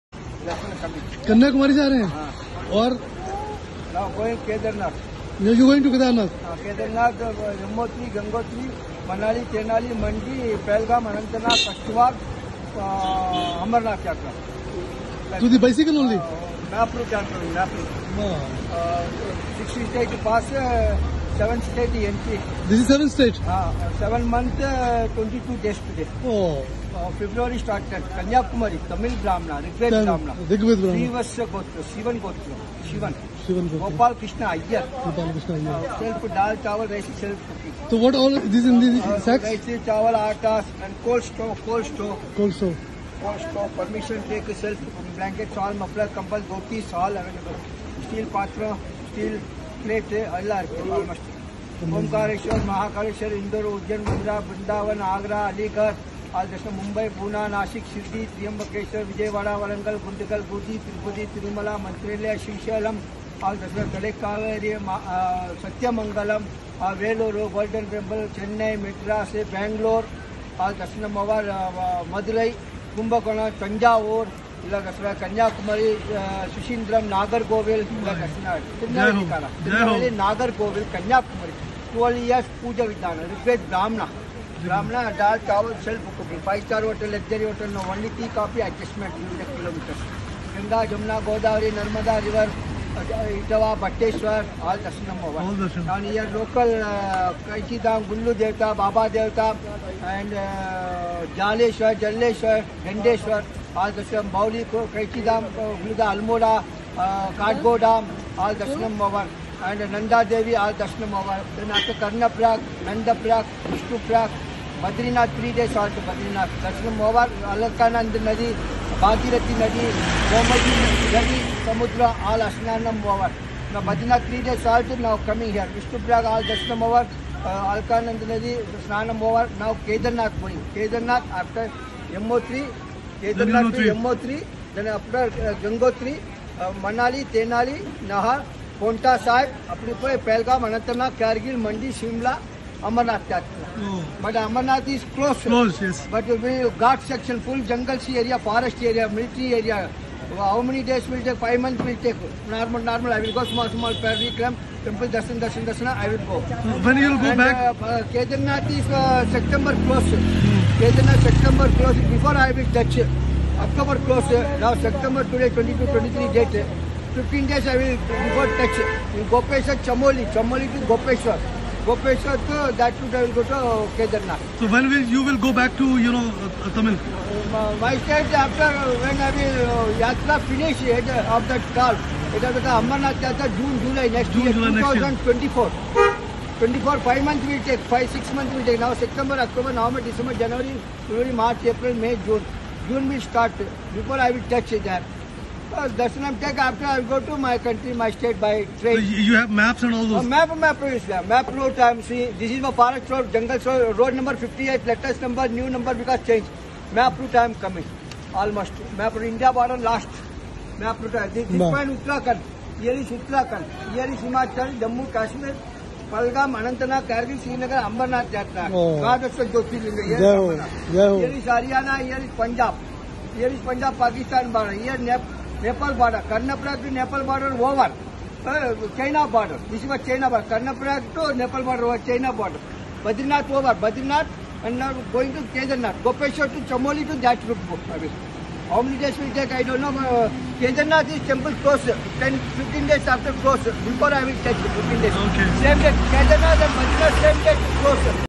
A pilgrim from Tamil Nadu going to Gangotri Carrying his ration for the journey.
He represents why Indian Civilization have survived.
It's so fulfilling Just to listen to the places he has visited.